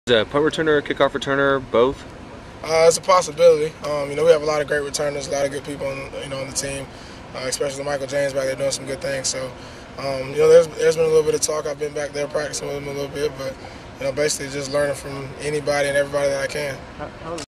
B.J. Daniels on possibly returning punts or kickoffs
I asked rookie quarterback B.J. Daniels, the San Francisco 49ers' seventh round pick, whether he might be asked to return punts or kicks.